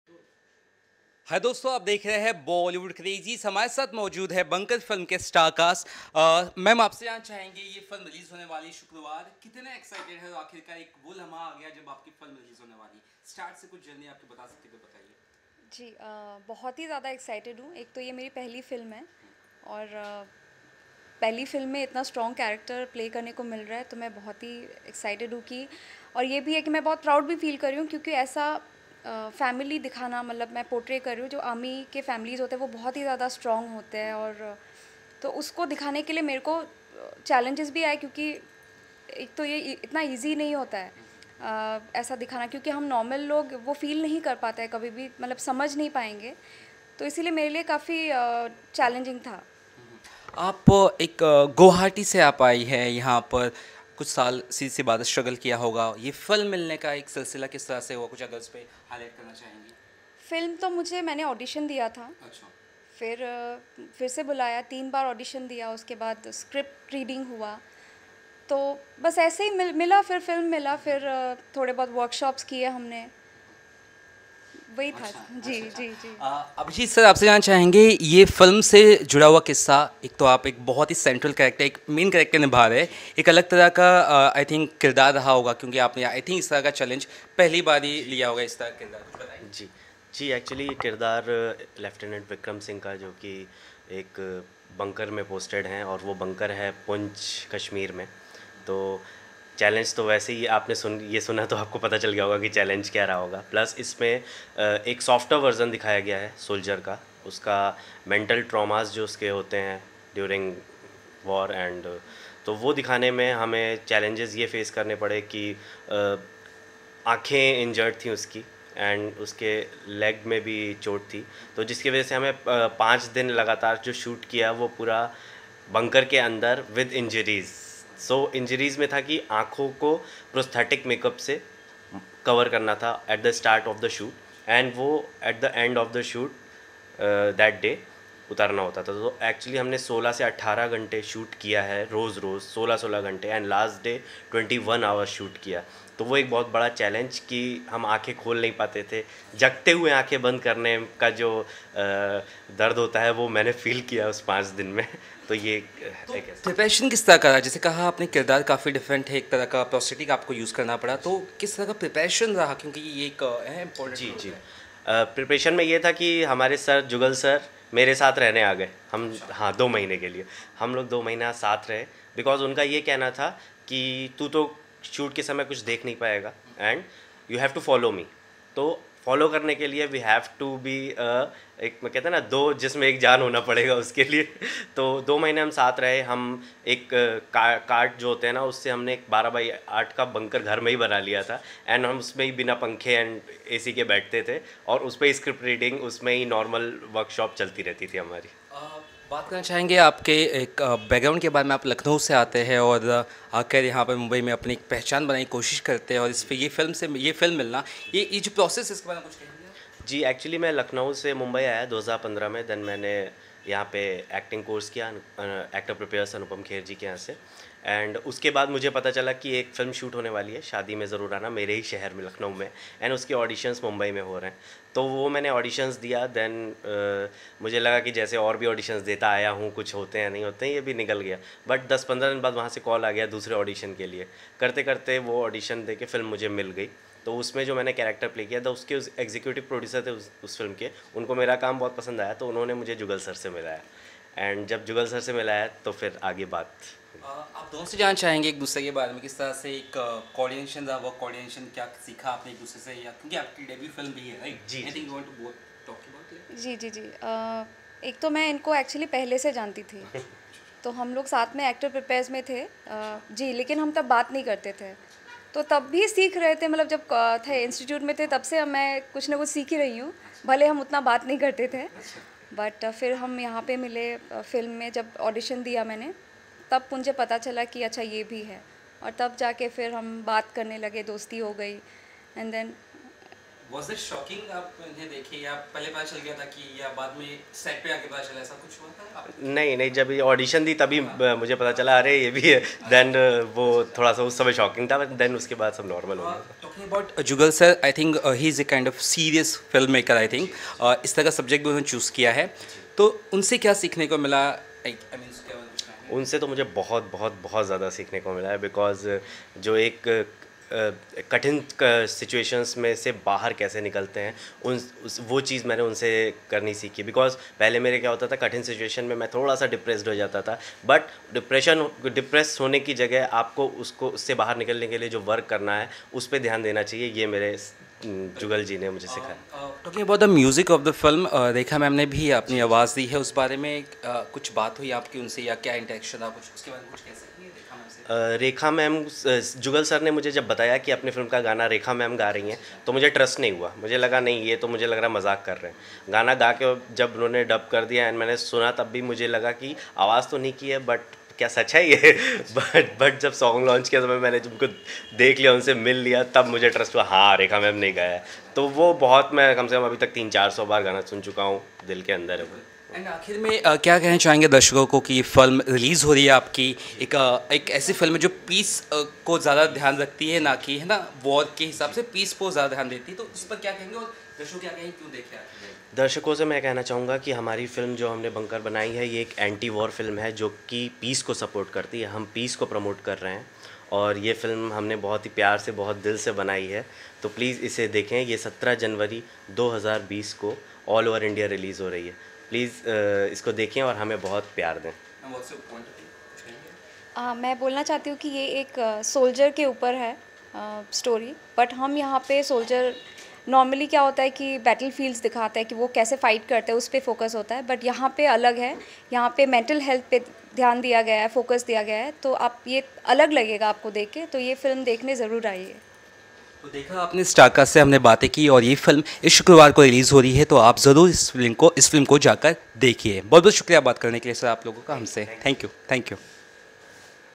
Actors Abhijeet Singh, Arindita Kalita & Director Jugal Raja talking about upcoming movie Bunker
Every soldier has a story to tell but he has to restrain his feelings and emotions. The soon to be released and India’s first Anti-War movie Bunker, is aimed to bring the stories of million soldiers to the masses. Director Jugal Raja's Bunker tells one such story of Lieutenant Vikram Singh (Played by actor Abhijeet Singh) as lone survivor with a fatal injury in a secret Bunker at the LOC in Poonch, Jammu, which has been hit by a mortar shell during a ceasefire violation.  The film has been showcased in numerous film festivals and was widely applauded for creating awareness on mental health issues amongst armed forces and their families.  In a philanthropic move, the makers of the film have announced 100% of the profits being donated to Bharat Ke Veer and Army Wives Welfare Association (AWWA) as homage to our armed forces. Movie is all set to release on 17th Jan 2020.